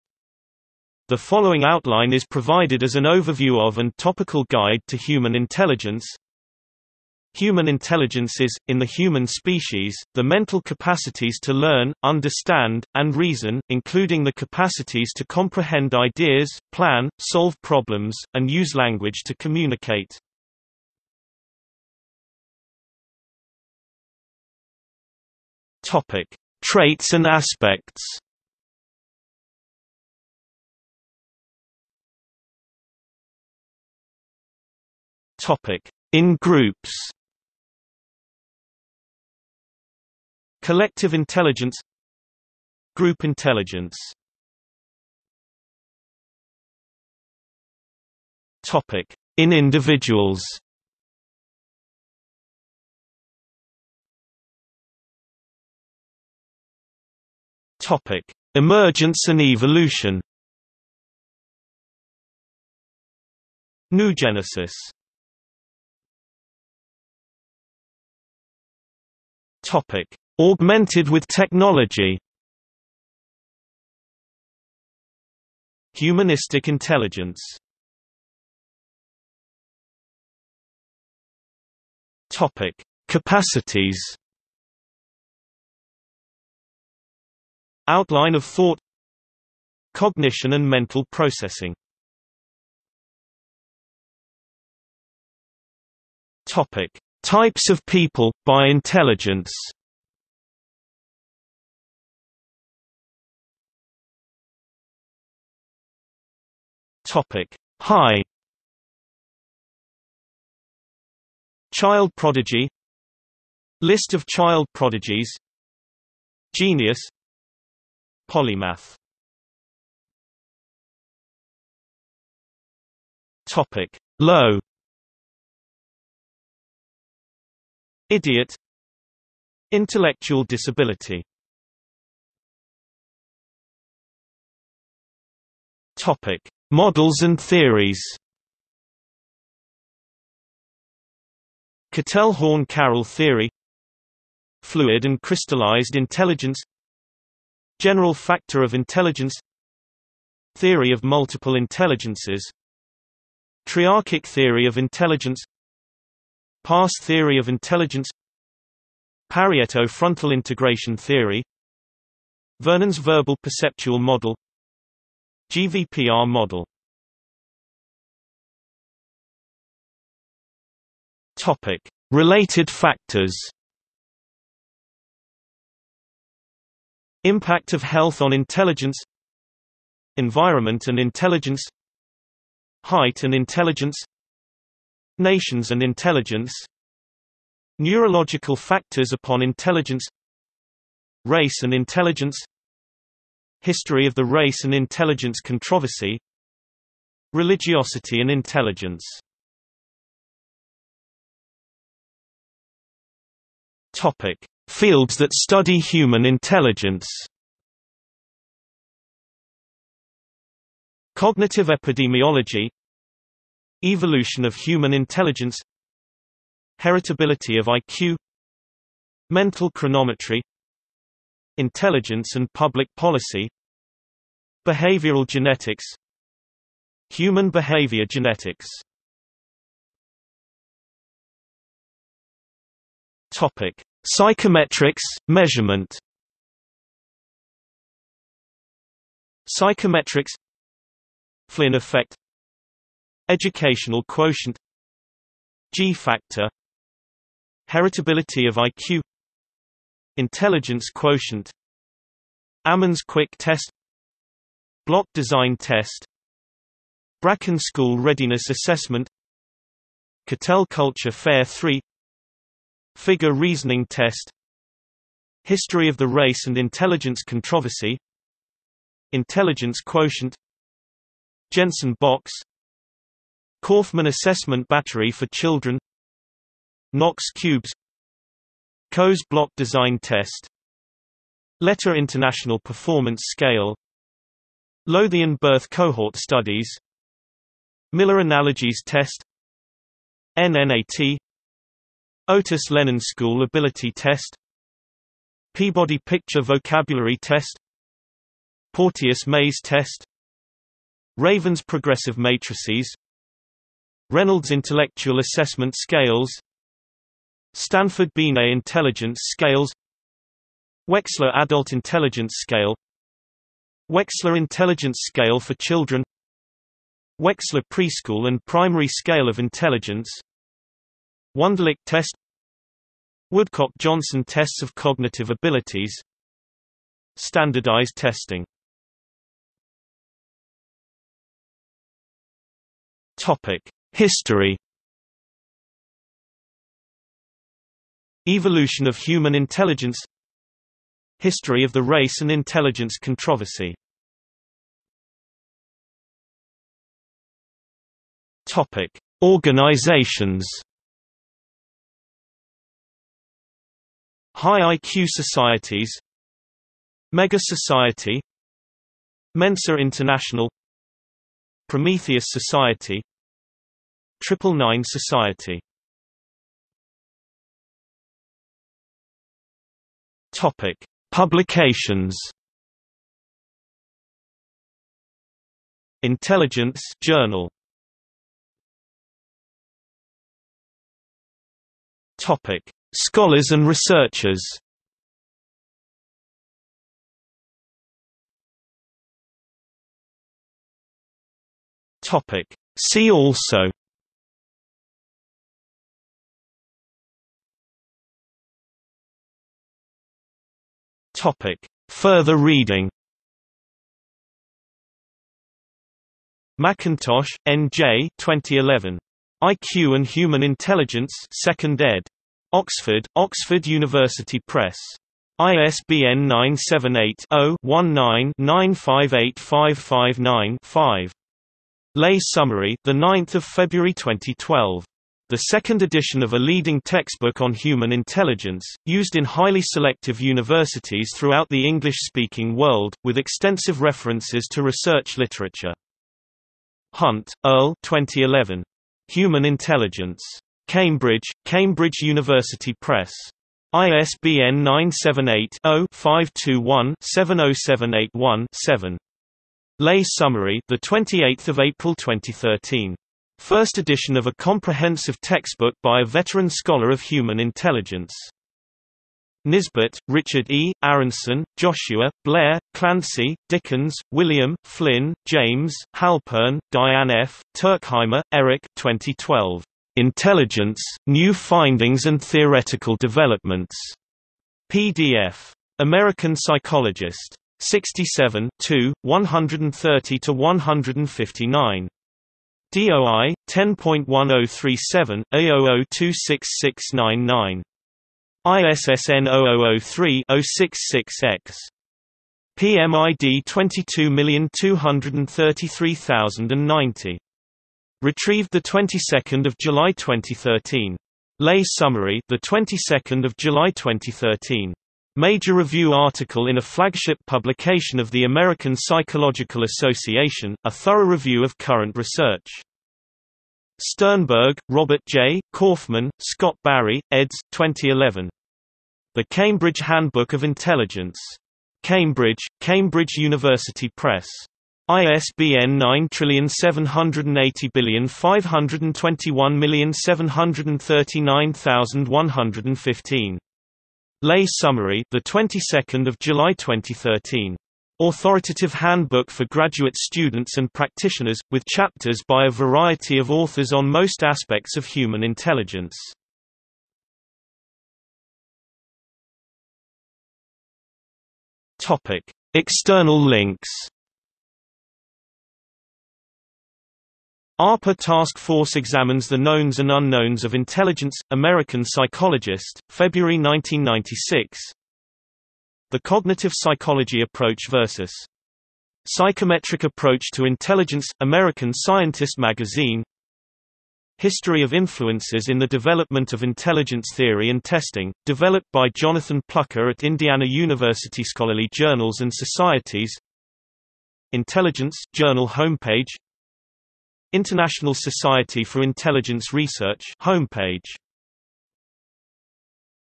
Outline of human intelligence | Wikipedia audio article
00:00:22 1 Traits and aspects
00:00:32 1.1 In groups
00:00:45 1.2 In individuals
00:00:54 2 Emergence and evolution
00:01:06 3 Augmented with technology
00:01:18 4 Capacities
00:01:32 5 Types of people, by intelligence
00:01:43 5.1 High
00:02:00 5.2 Low
00:02:12 6 Models and theories
00:02:47 7 Related factors
00:03:20 8 Fields that study human intelligence
00:03:49 8.1 Psychometrics: measurement
00:05:29 9 History
00:05:44 10 Organizations
00:06:06 11 Publications
00:06:18 12 Scholars and researchers
00:06:28 13 See also
00:06:37 14 Further reading
00:09:08 15 External links

- Socrates

SUMMARY
The following outline is provided as an overview of and topical guide to human intelligence:
Human intelligence is, in the human species, the mental capacities to learn, understand, and reason, including the capacities to comprehend ideas, plan, solve problems, and use language to communicate.